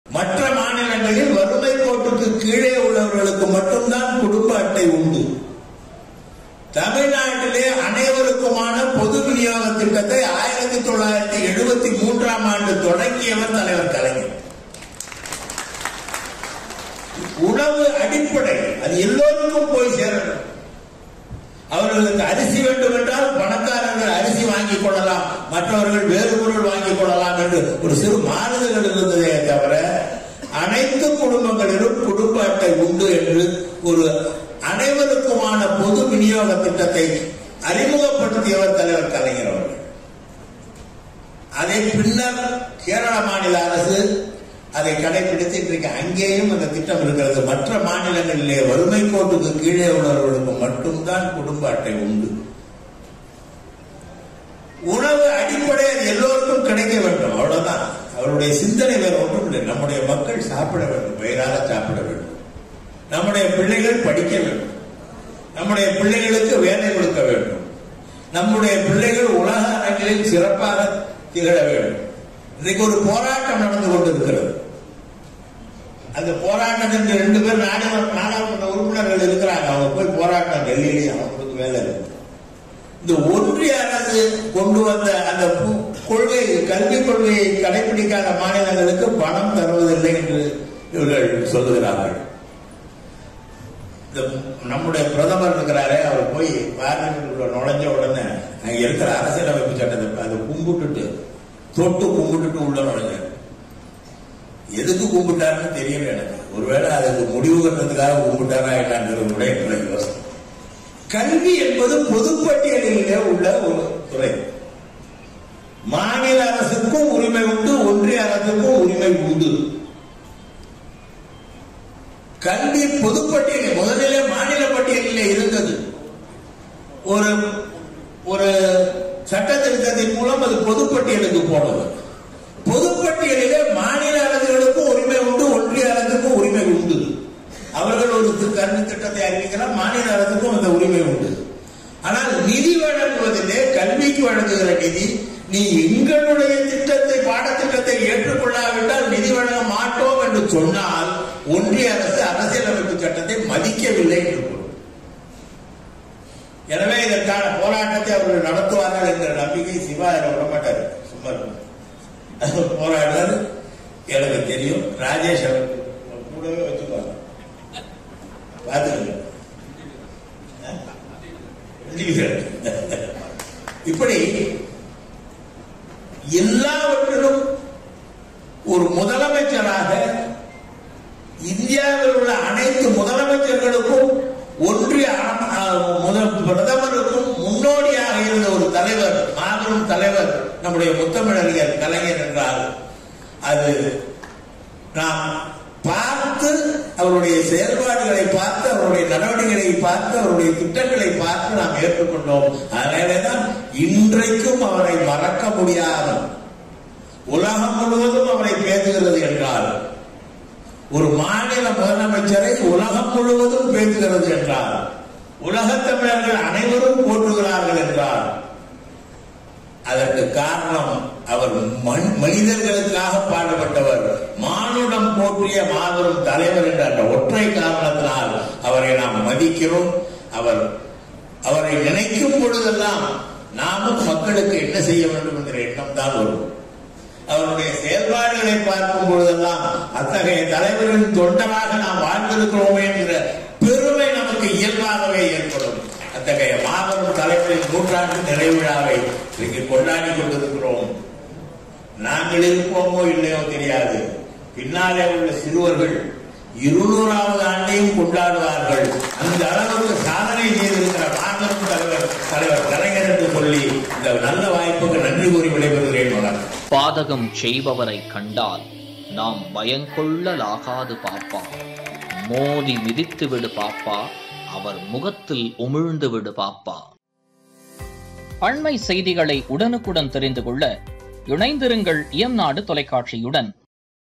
TKS Elangovan :கலைஞர் கொண்டுவந்த திட்டம்... இதுவரை எந்த மாநிலத்திலும் கிடையாது - டி.கே.எஸ் இளங்கோவன்
TKS Elangovan Speech :கலைஞர் கொண்டுவந்த திட்டம்... இதுவரை எந்த மாநிலத்திலும் கிடையாது - டி.கே.எஸ் இளங்கோவன்